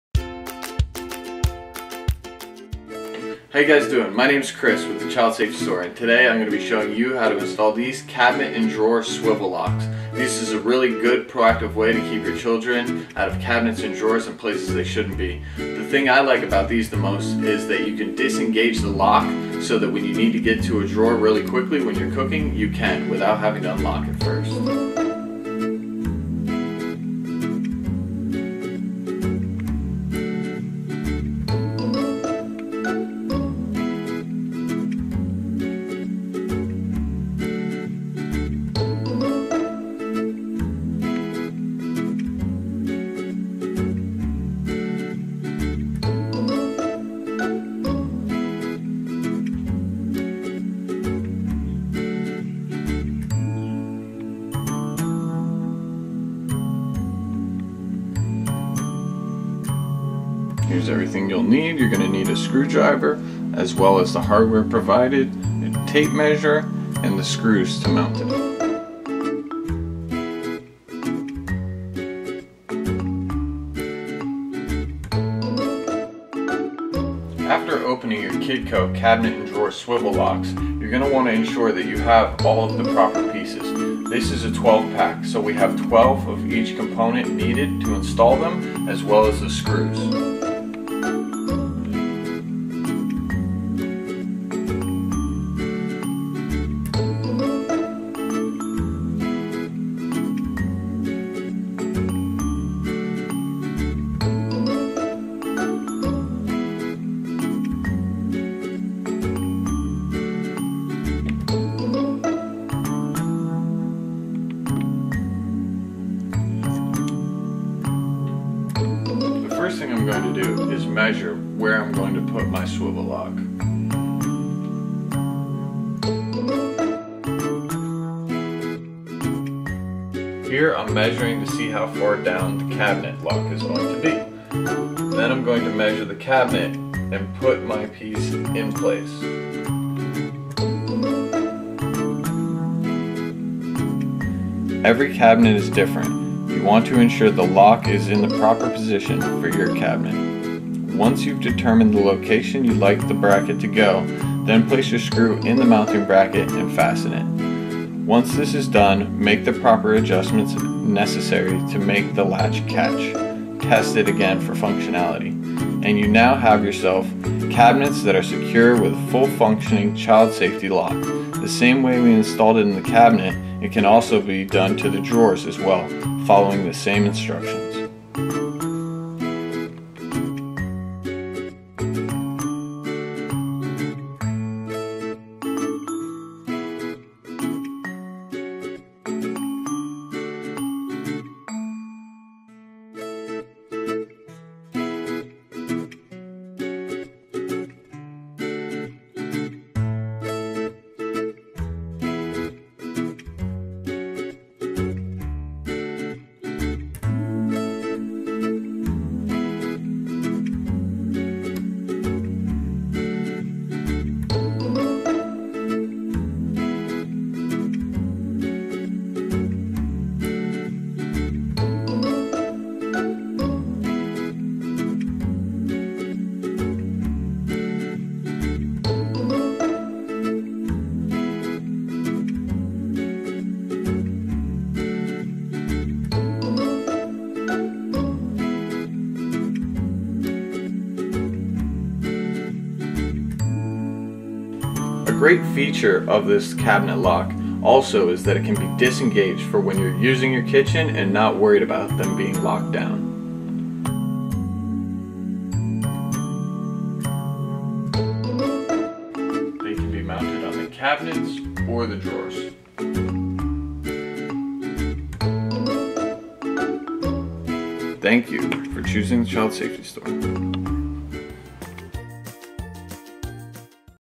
Kidco Cabinet and Drawer Swivel Locks Installation Video
Today our child safety expert Chris Lupton will walk you through, step-by-step, in installing Kidco's Cabinet and Drawer Swivel Locks.